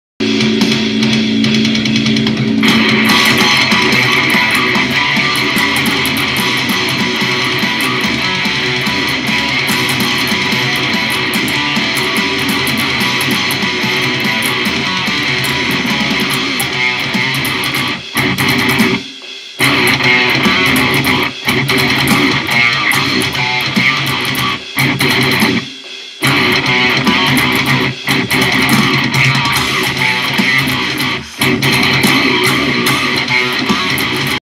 Feel The Groove
Jamming through something i written for my band!